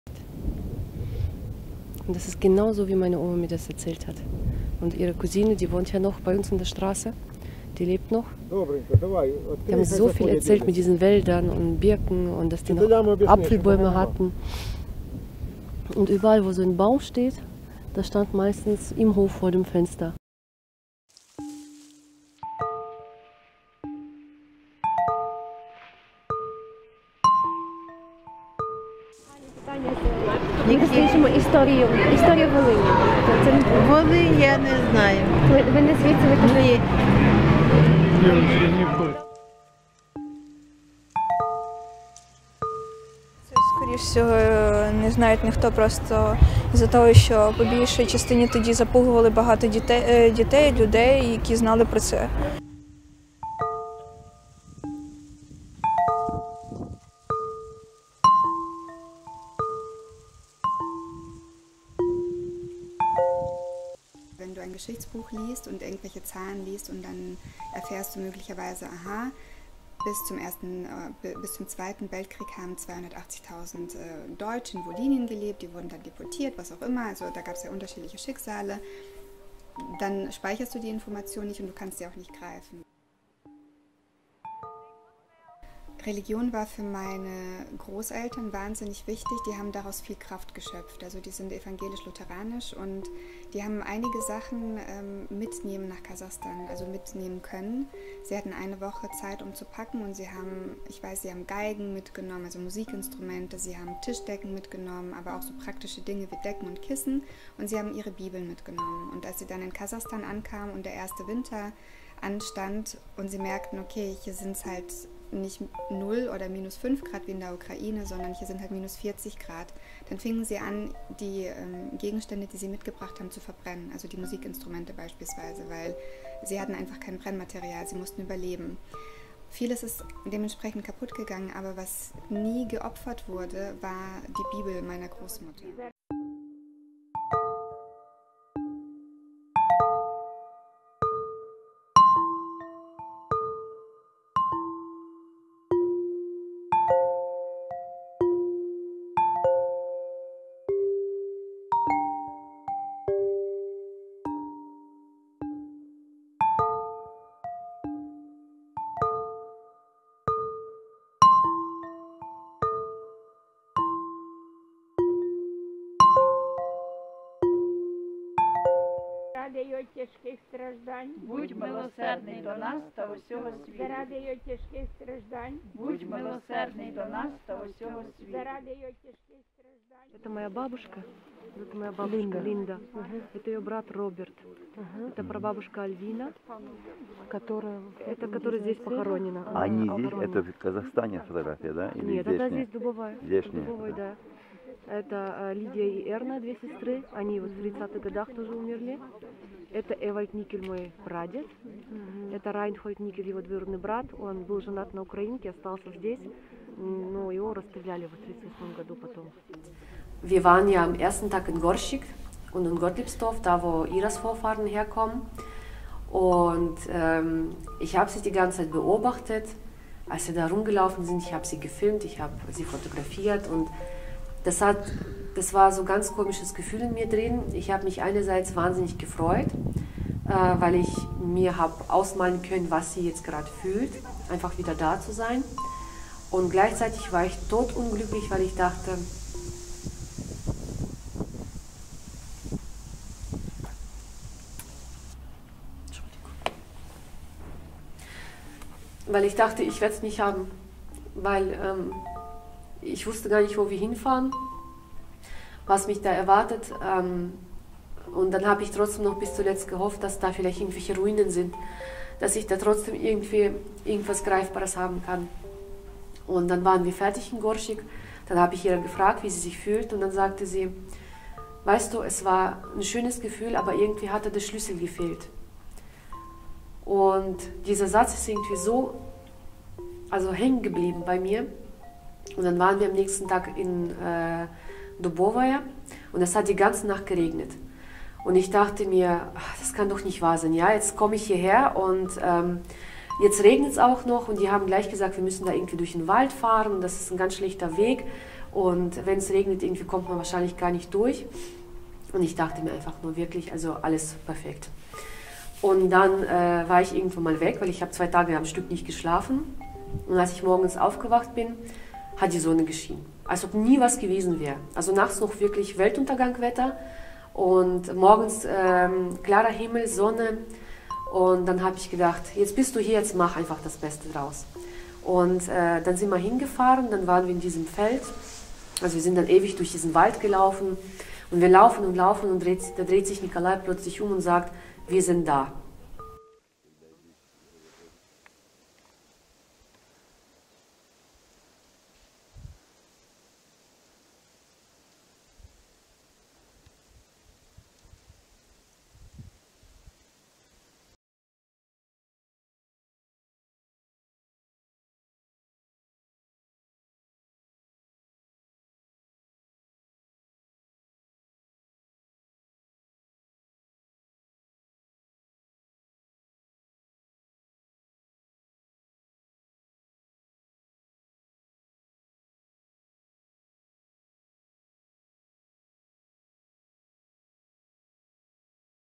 Das, was bleibt. Eine Spurensuche in Wolhynien
Im Zuge der Recherchen zur „Medien- und Autorenwerkstatt Deutsche in Wolhynien“ setzten sich Nachwuchsjournalistinnen und -journalisten aus Deutschland und der Ukraine mit der Geschichte der Wolhyniendeutschen auseinander. Unser Kollege Helge Theil war dabei: In der Oblast Schytomyr begab er sich im Herbst 2019 auf die Spuren der Deutschen, die bis zum Zweiten Weltkrieg dort siedelten. Am Höhepunkt der relativ kurzen Siedlungsphase, kurz vor dem Ersten Weltkrieg, wurde ihre Zahl auf bis zu 280 000 geschätzt.
Der Film ist eines der Produkte der Filmgruppe, in der unser Kollege mitgearbeitet hat.

Konzept: Piotr Armianovski, Saskia Heller, Yaroslava Hryhorchuk, Yulia Saidel, Helge Theil
Kamera: Piotr Armianovski, Milan Bath, Yaroslava Hryhorchuk, Paul Toetzke
Schnitt: Piotr Armianovski, Saskia Heller, Yaroslava Hryhorchuk, Helge Theil
Ton: Saskia Heller, Helge Theil
Technische Nachbearbeitung: Helge Theil
Interviews: Helge Theil

Das Projekt wurde durch Mittel der Bundesbeauftragten für Kultur und Medien ermöglicht.